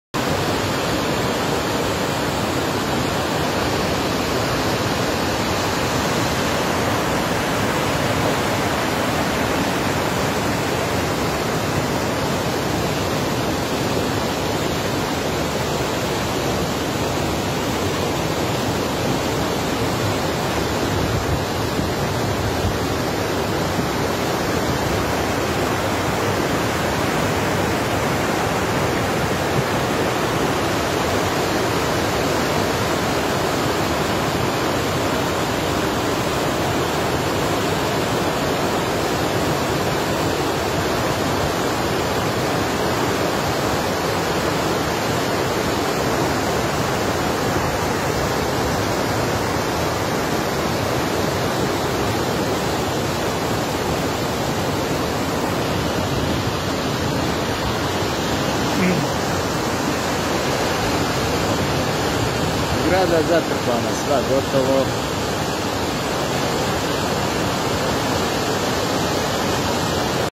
Drinjača uništava mini centralu kod Kamenice u Glodima
Drinjača uništava izgradjenu mini centralu u mjestu Glodi